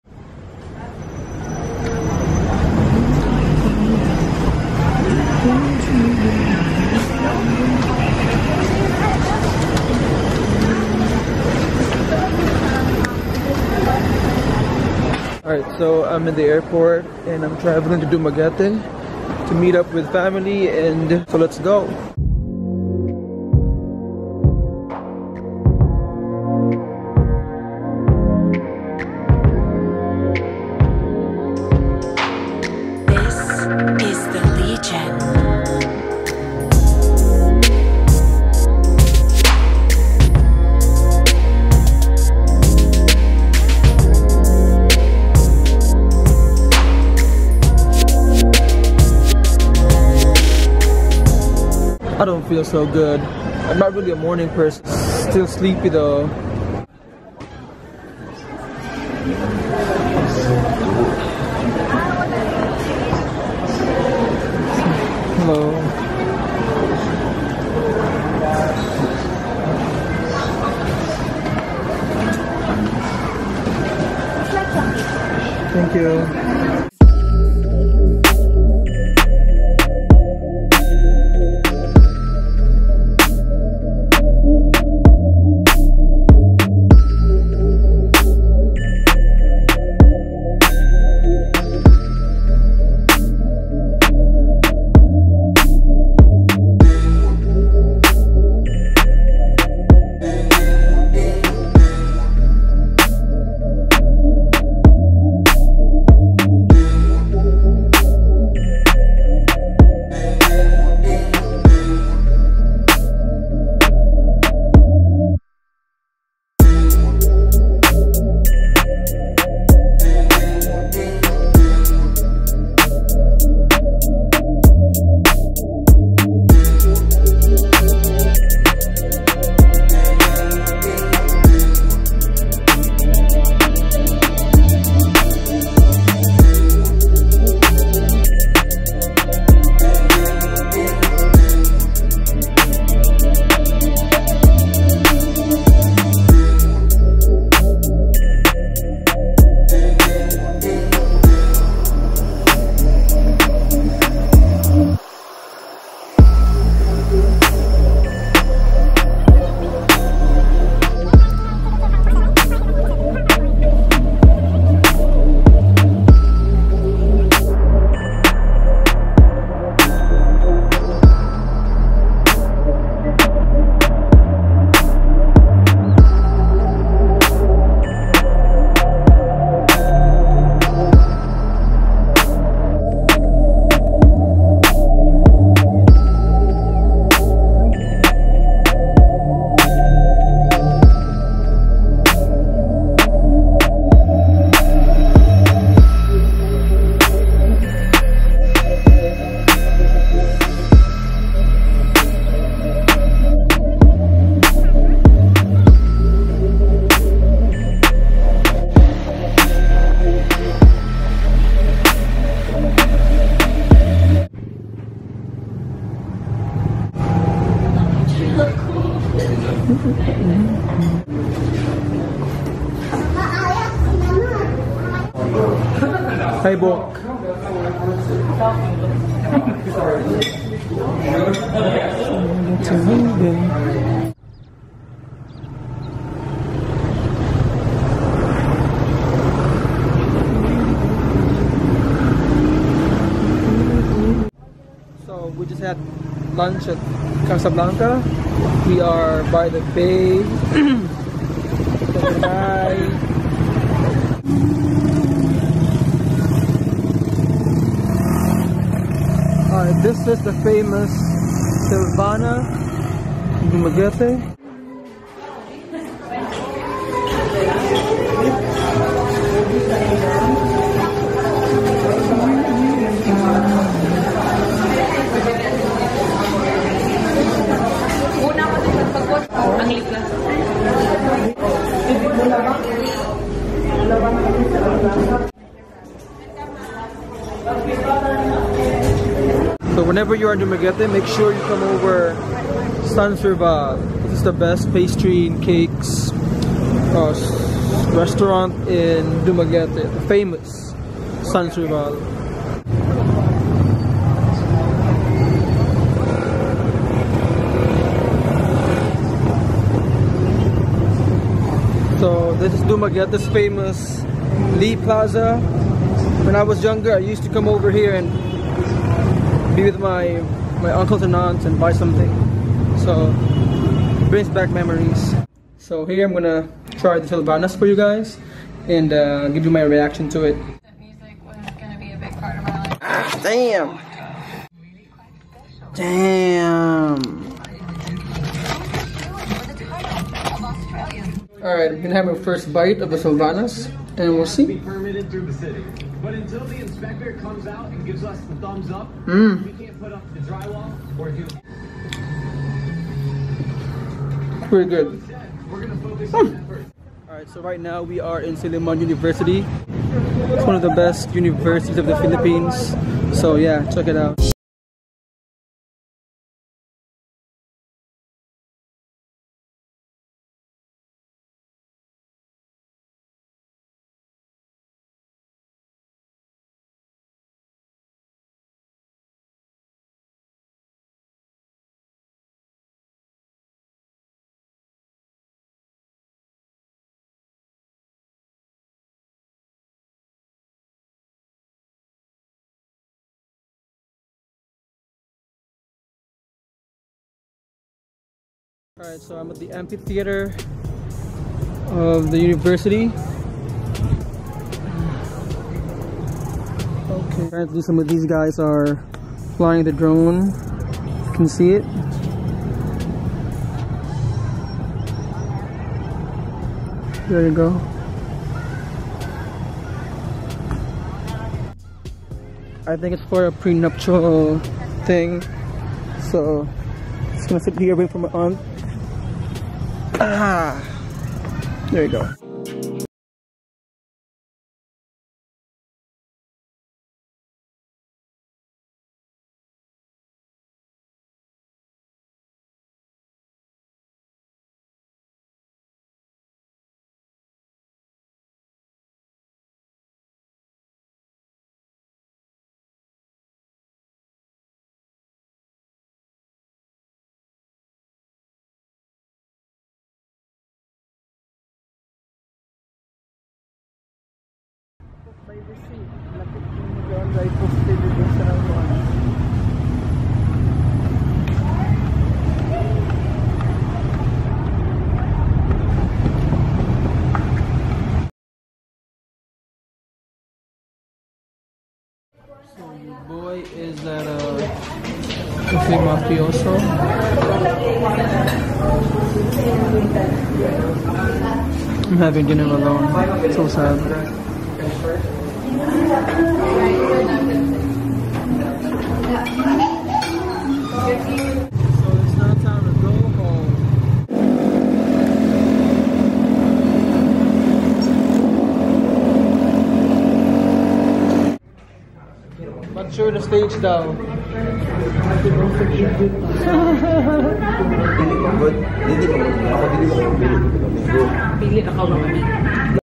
Vlog 5: Discovering Dumaguete (Sans Rival, Checking Silliman University)
flew out to Dumaguete to visit cousins. Tag along and let me show you some sights around the city and some of the best restaurants. :)